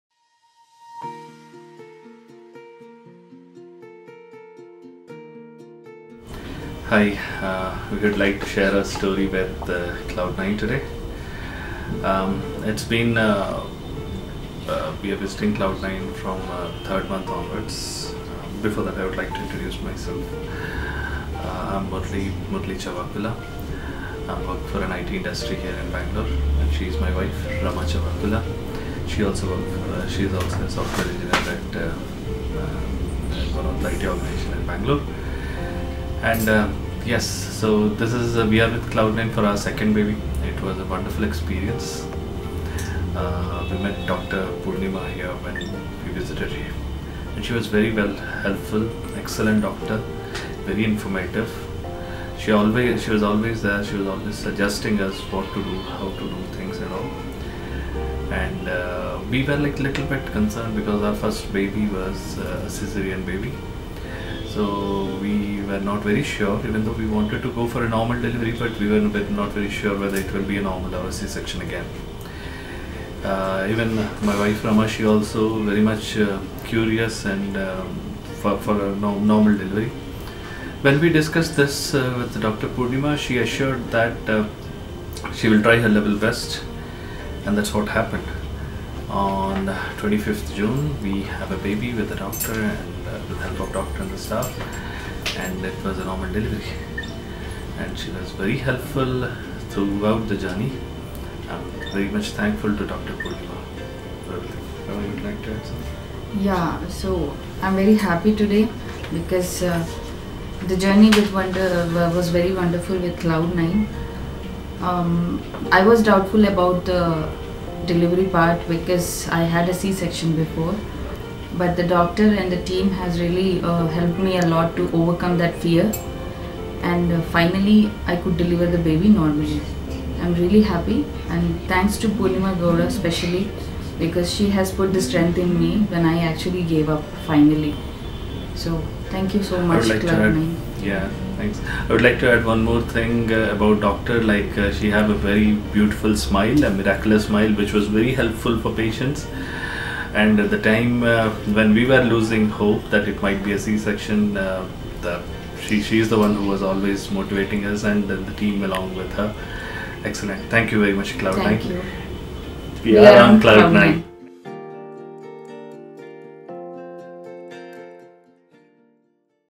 Rama & Murali | Cloudnine Hospitals | Reviews | Testimonials
Nothing makes us happier than a satisfied customer! Meet Mr. Murali Krishna and Mrs. Rama Murali Krishna - two of our many happy customers. Watch this video & get to know their experience with Cloudnine Hospitals. :)

Cloudnine Hospitals
Healthy mothers. Happy babies.